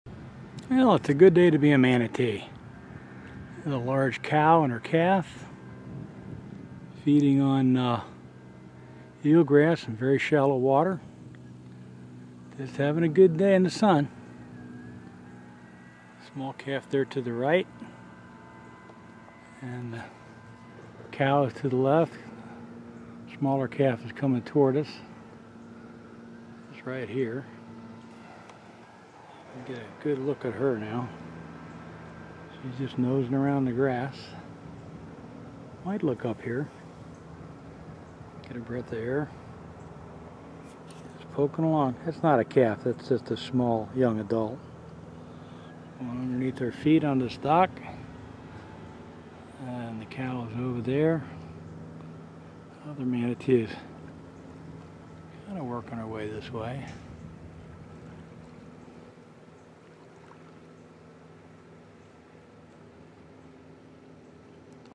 Good Day To Be A Manatee
Warm summer weather is just right for a manatee and her large calf to feed on eel grass in shallow Florida water.